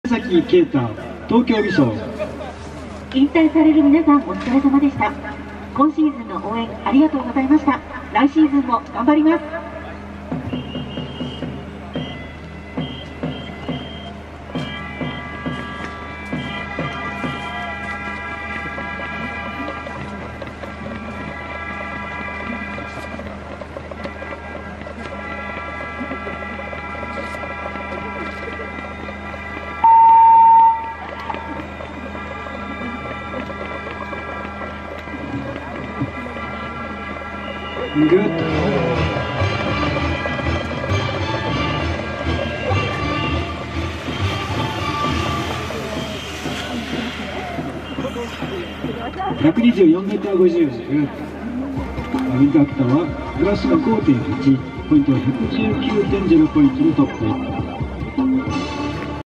宮﨑敬太 選手 第24回伊藤杯シーズンファイナル １本目 124.5メーター 2023/03/18(土) 大倉山ジャンプ競技場
東京美装のケイタ選手。１本目６位。